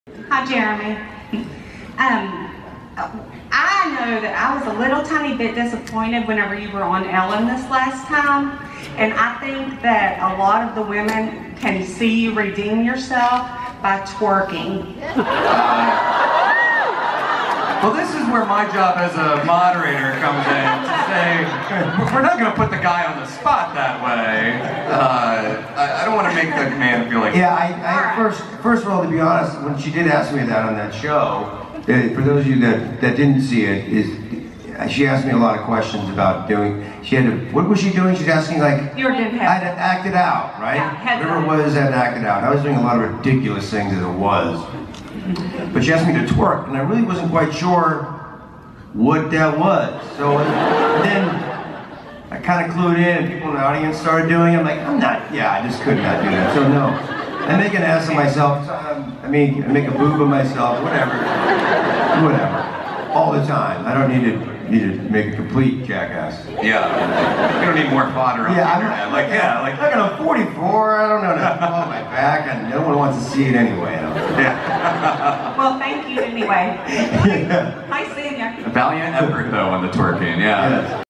Jeremy Renner - Reasons Not To Twerk (Chicago Wizard World 2015)
Jeremy Renner at Chicago Wizard World Comic Con on August 23rd, 2015.

A fan asks Jeremy why he wouldn't "twerk" when asked to on Ellen earlier in the year.